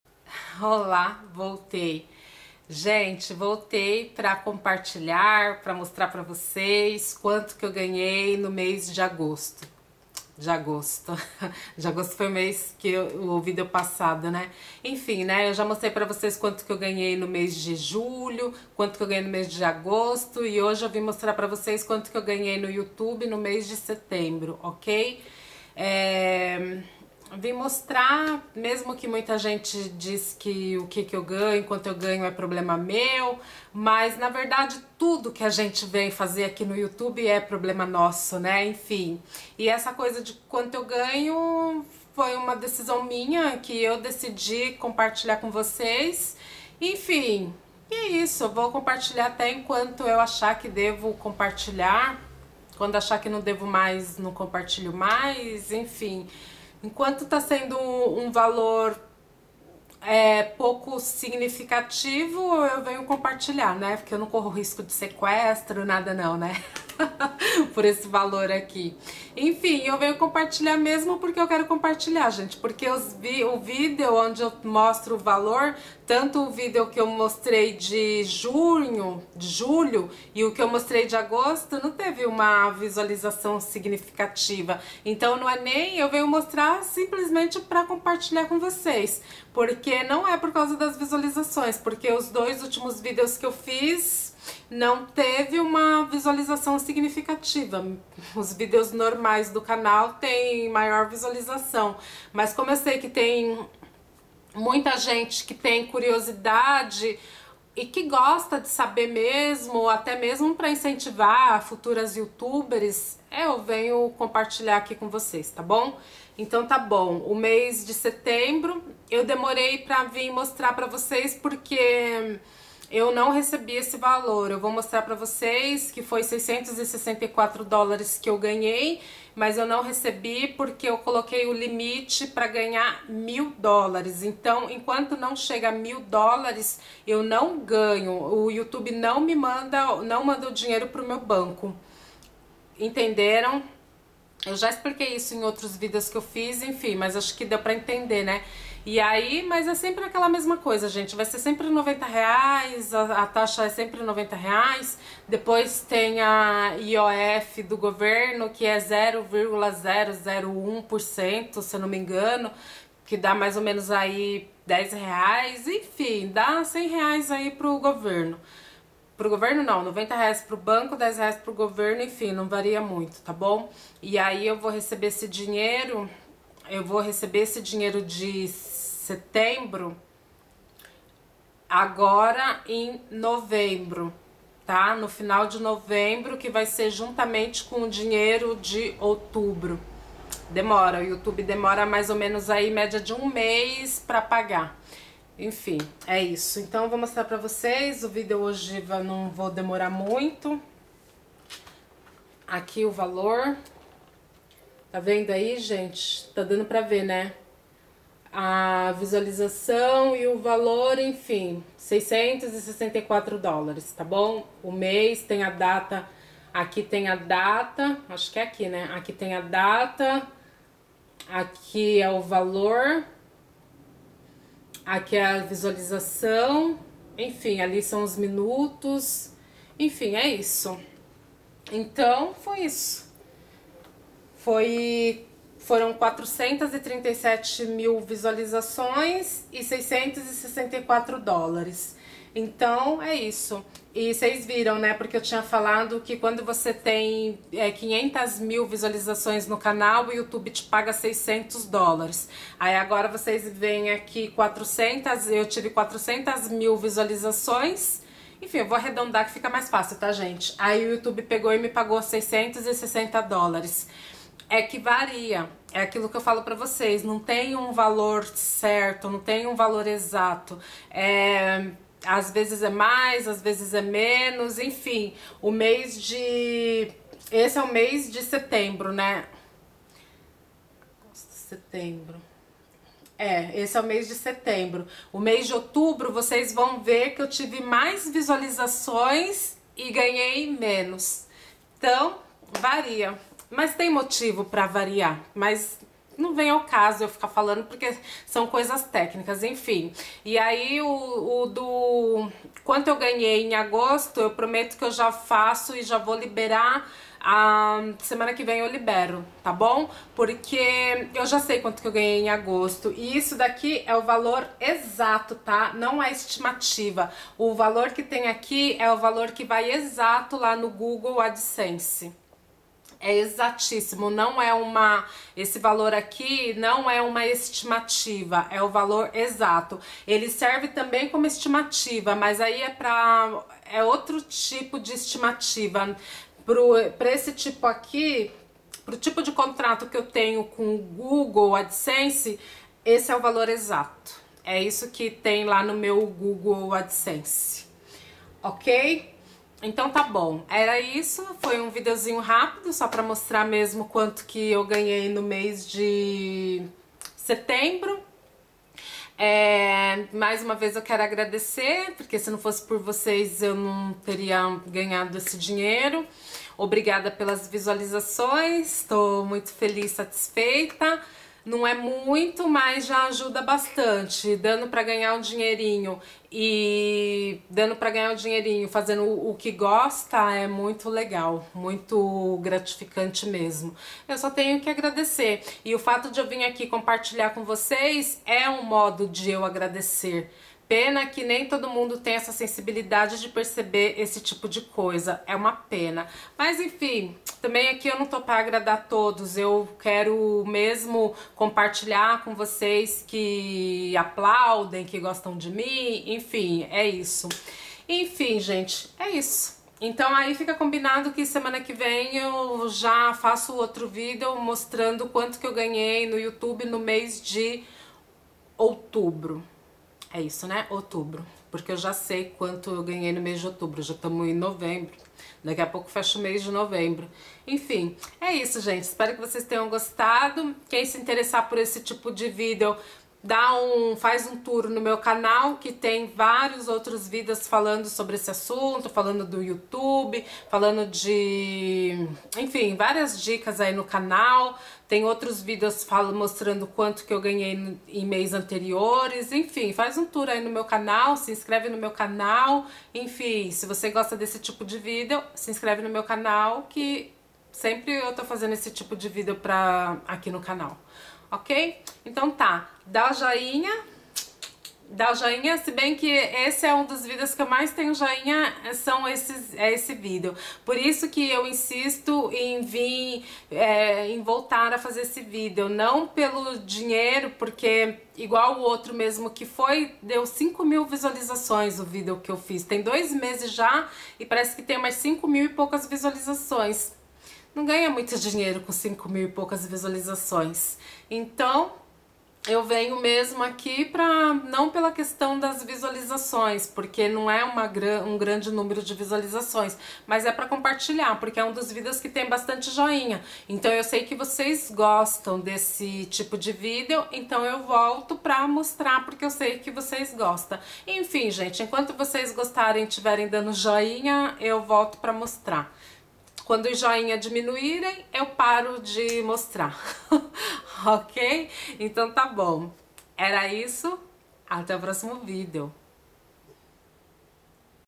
Qto Ganhei no Youtube em Setembro com o Canal.
Quanto ganhei no Google Adsense. Será q o Youtube Paga Bem?
Neste vídeo mostro quantas visualizações o meu canal teve no Mês de Setembro e quantos dolares, dinheiro eu ganhei.